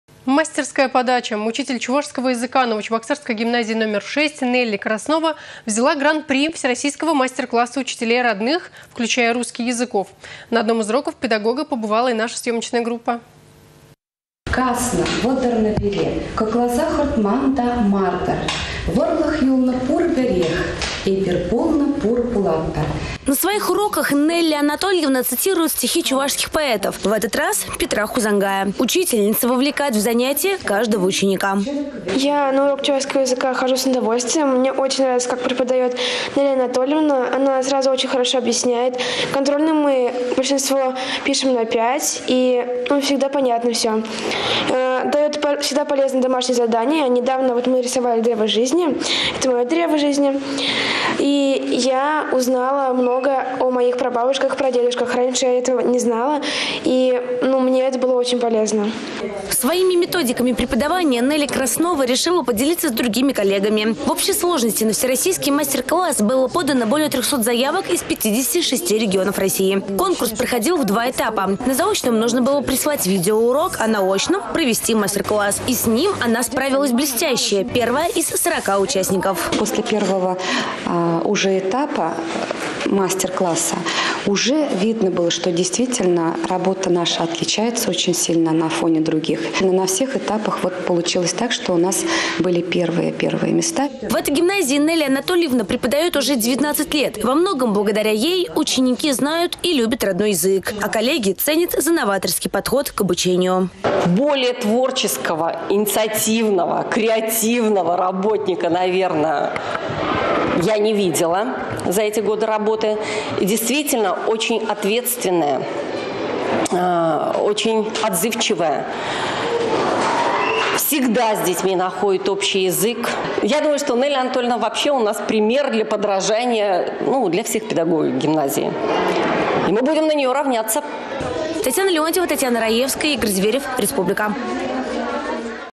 Учитель новочебоксарской гимназии — выиграла гран-при Всероссийского конкурса учителей
Мастерская подача. Учитель чувашского языка новочебоксарской гимназии № 6 Нелли Краснова взяла Гран-при Всероссийского мастер-класса учителей родных, включая русский, языков. На одном из уроков педагога побывала и наша съемочная группа.